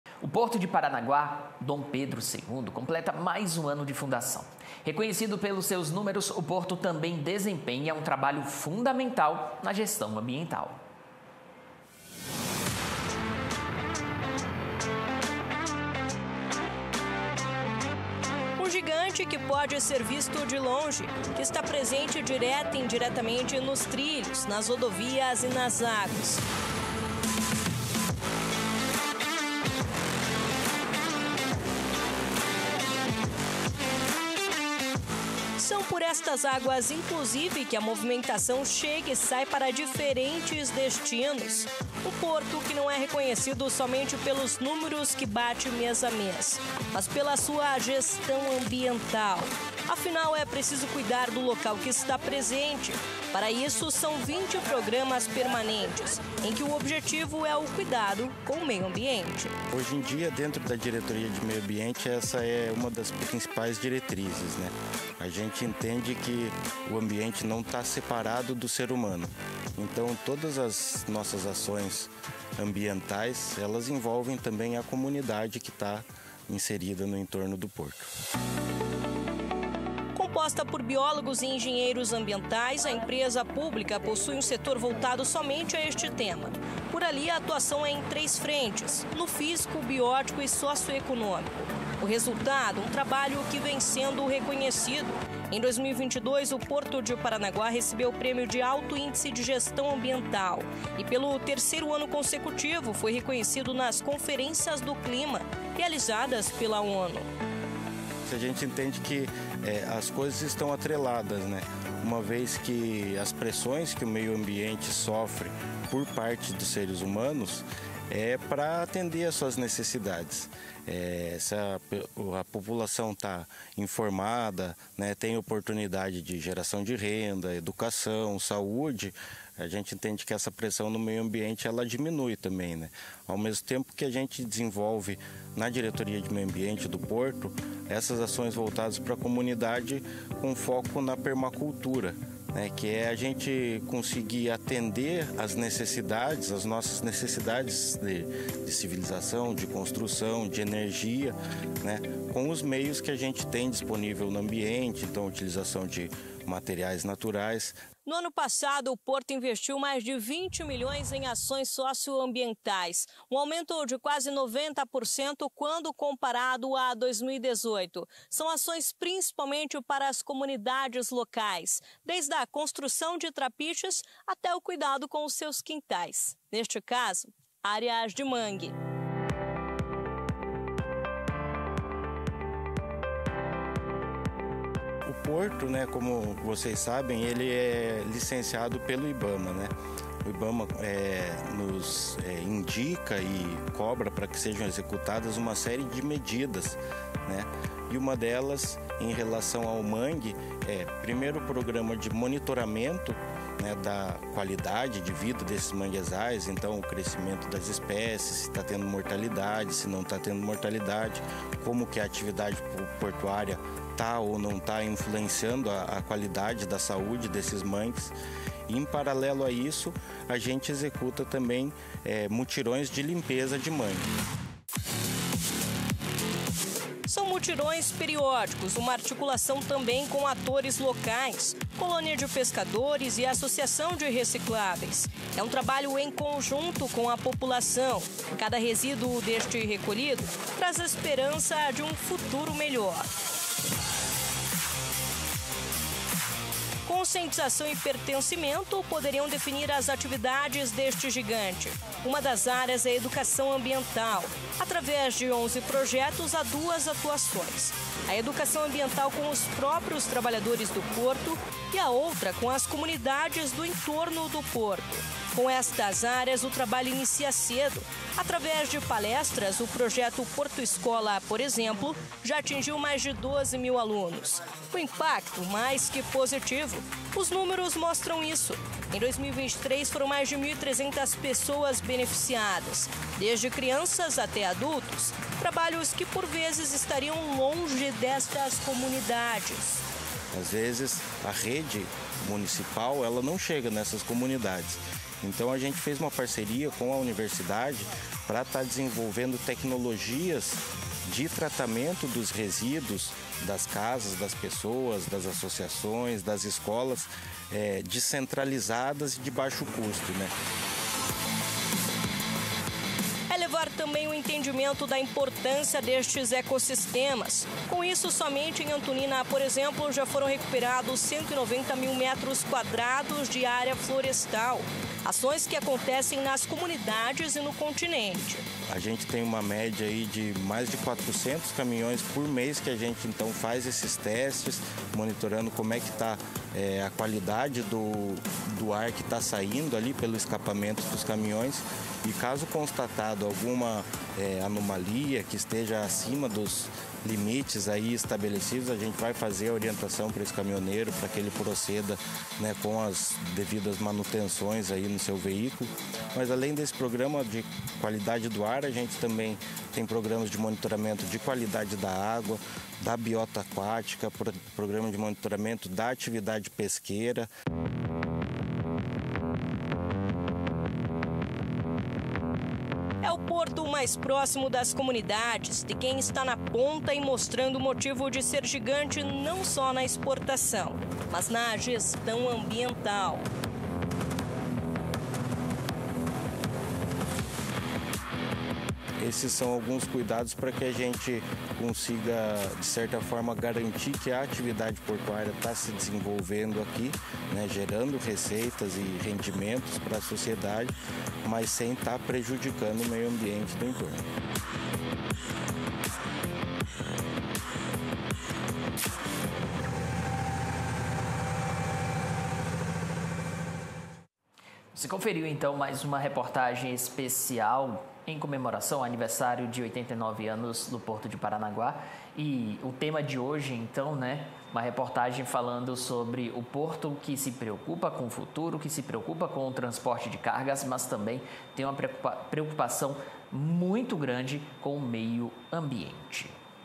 ANIVERSÁRIO DO PORTO DE PARANAGUÁ
O porto famoso por seus recordes batidos anualmente, também se destaca pela gestão ambiental